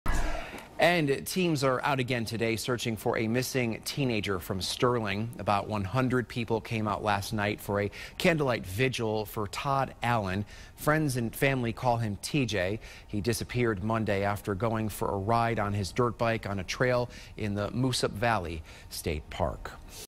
Authorities search a fifth day for missing dirt biker
Connecticut authorities are searching for a fifth day for a dirt biker who disappeared Monday after texting his girlfriend that he had fallen off his motorcycle and hurt his leg.